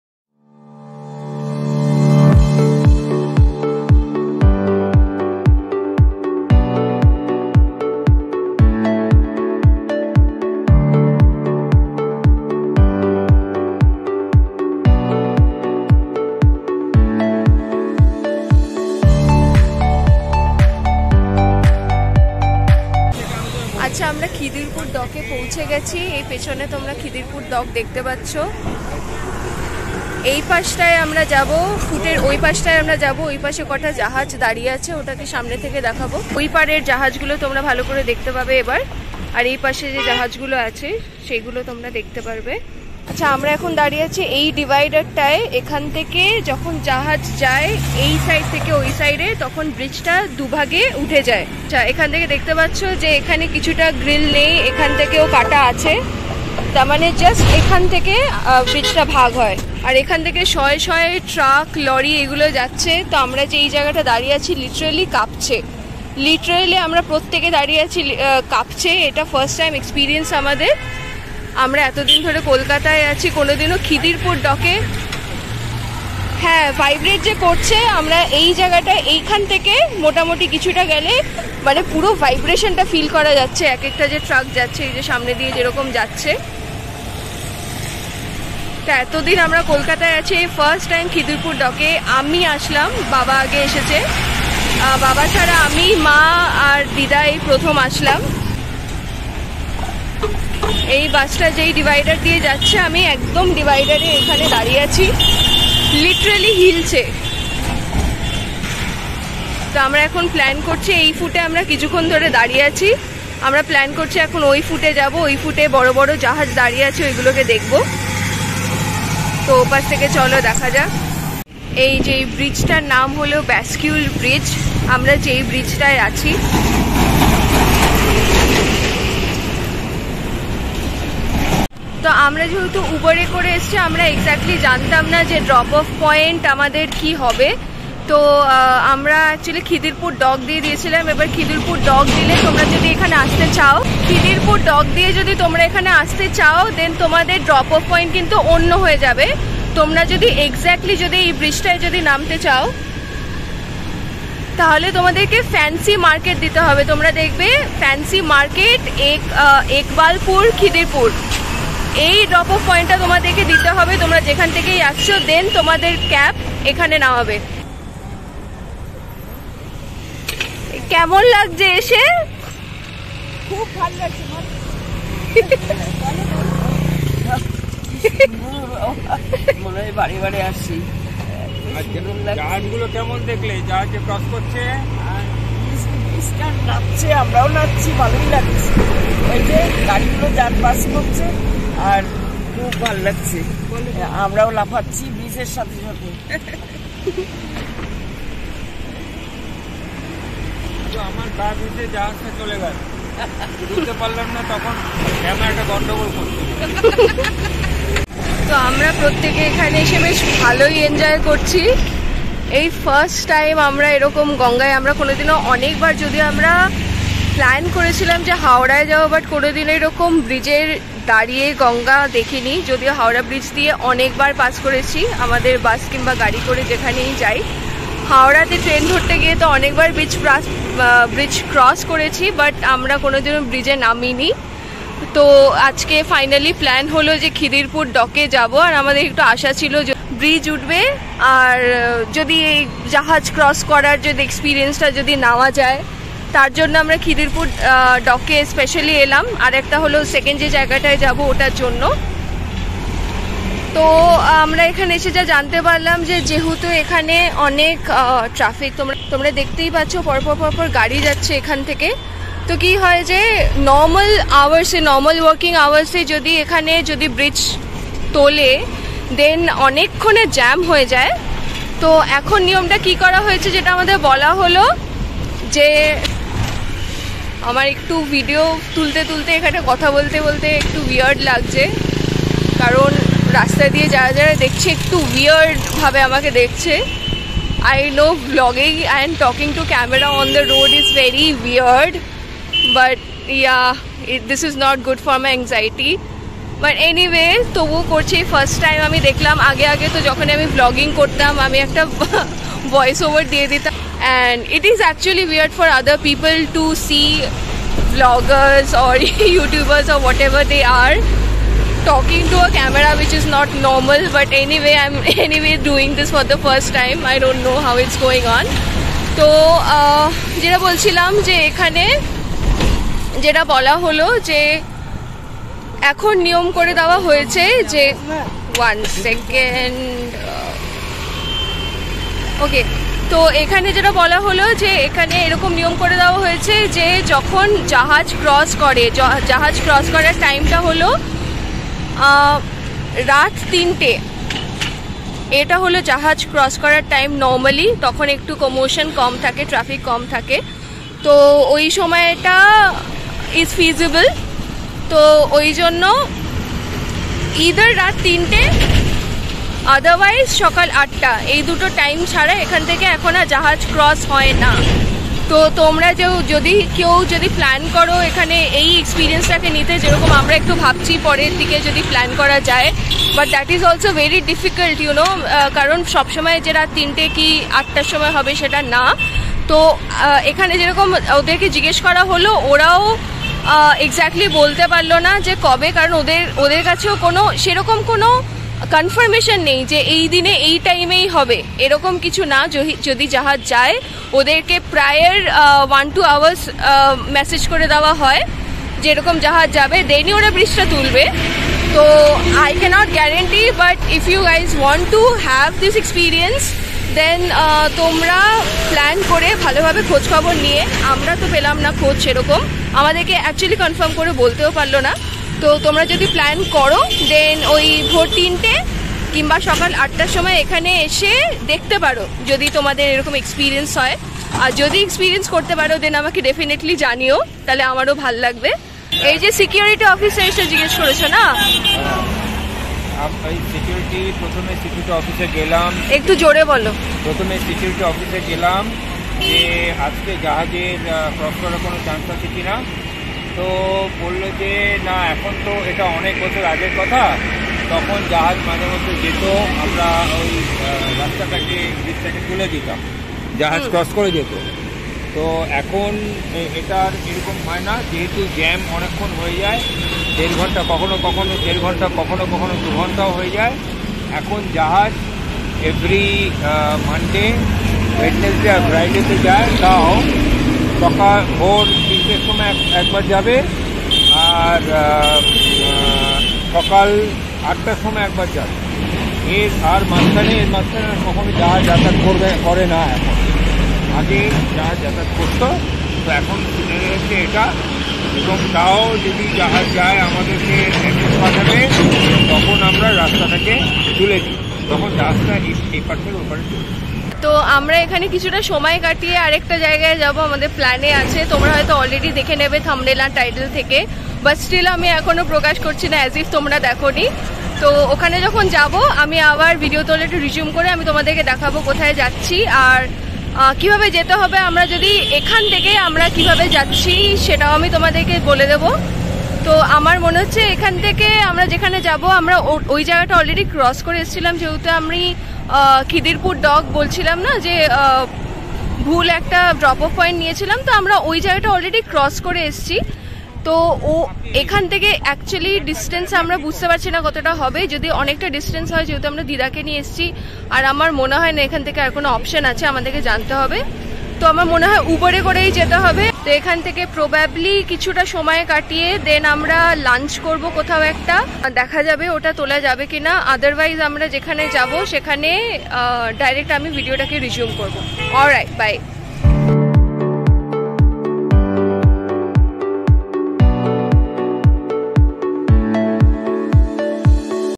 Kidderpore Dock Vlog | Kidderpore Folding Bridge | Bascule Bridge | Kolkata | Suchismita
Hey Guys! I am back with a Vlog on my channel after a long time. Hope you enjoy watching it.
Love ya always xoxo...